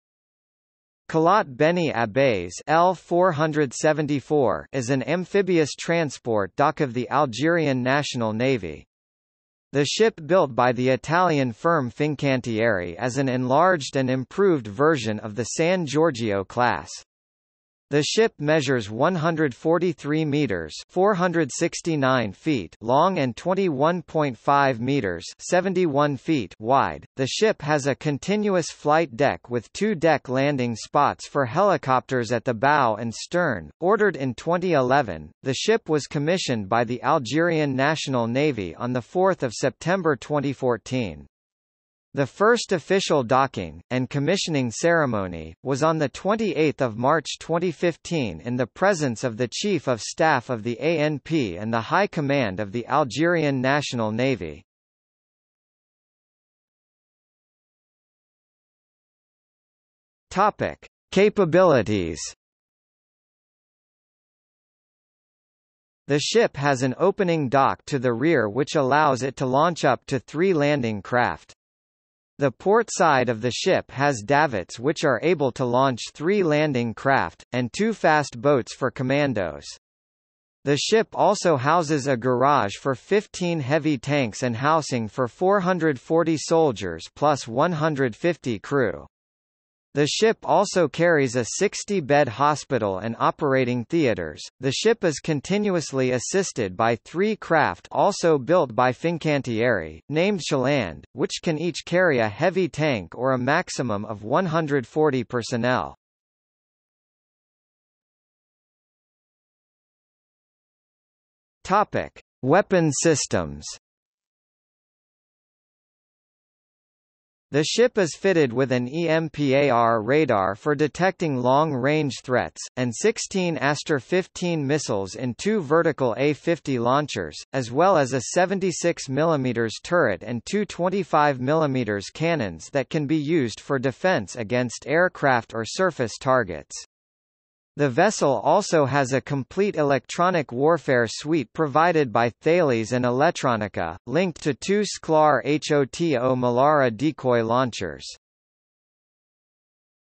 Algerian amphibious transport dock Kalaat Béni Abbès (474) | Wikipedia audio article
00:00:56 Capabilities
00:01:48 Weapon systems

- Socrates

SUMMARY
Kalaat Beni Abbes (L-474) is an amphibious transport dockof the Algerian National Navy. The ship built by the Italian firm Fincantieri as an enlarged and improved version of the San Giorgio class. The ship measures 143 metres (469 ft) long and 21.5 metres (71 ft) wide.The ship has a continuous flight deck with two deck-landing spots for helicopters at the bow and stern.Ordered in 2011, the ship was commissioned by the Algerian National Navy on 4 September 2014. The first official docking - and commissioning ceremony - was on 28 March 2015 in the presence of the Chief of Staff of the ANP and the High Command of the Algerian National Navy.